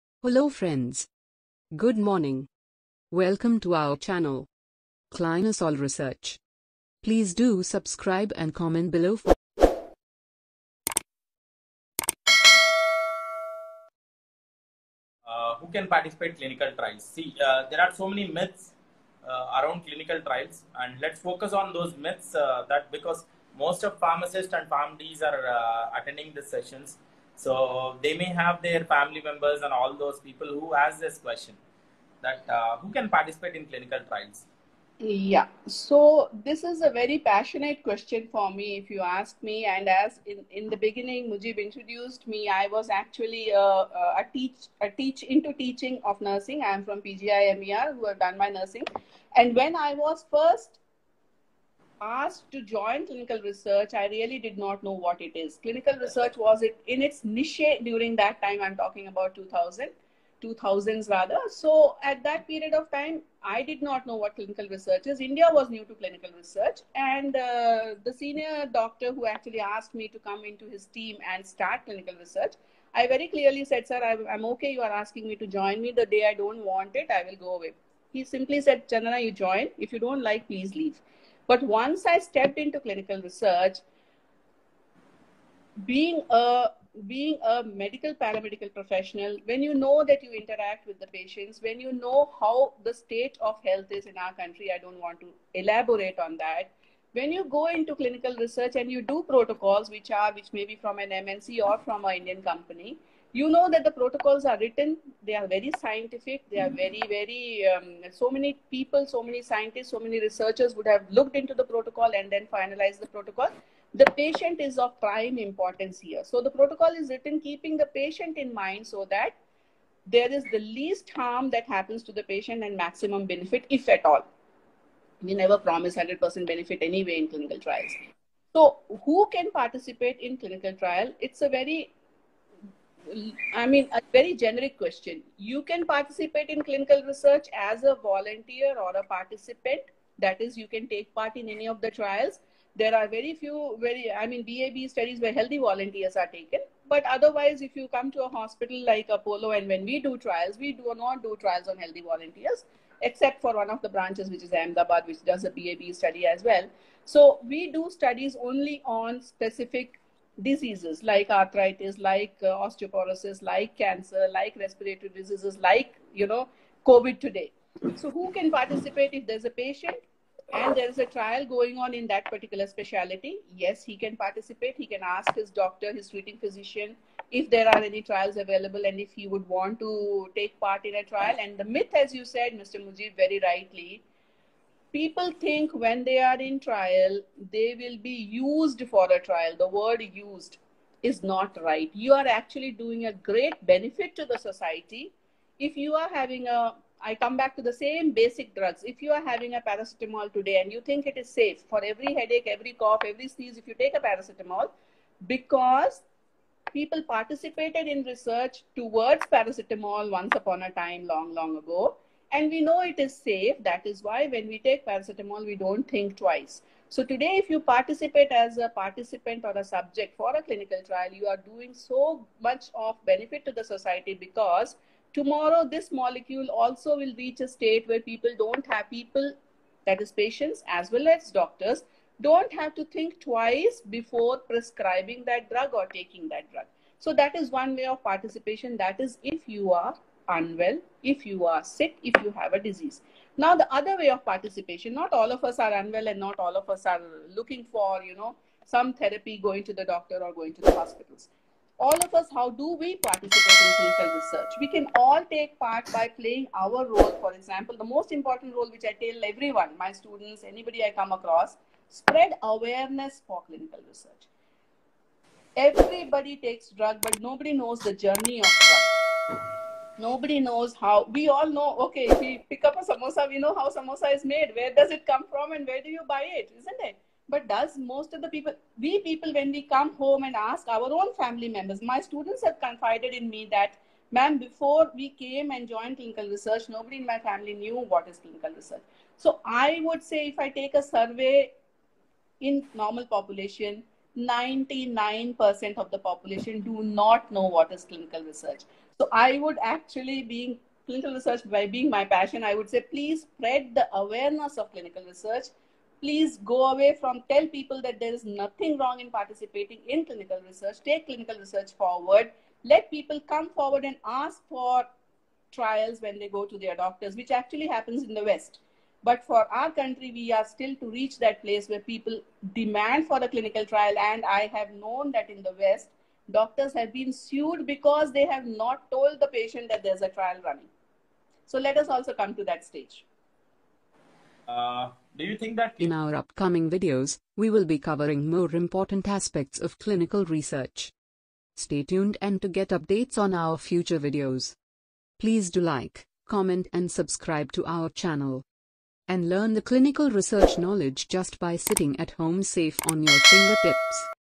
who can participate in clinical trials?| Is clinical research right for you|Clinosol research Part 3
Every disease treatment prescribed today is the result of clinical research. These discoveries are not possible without volunteers like you!

Are you really interested to know who can participate then do watch this video..

"Join Clinosol and secure your career in clinical research Industry for your brighter future."

For more information and updates do follow us on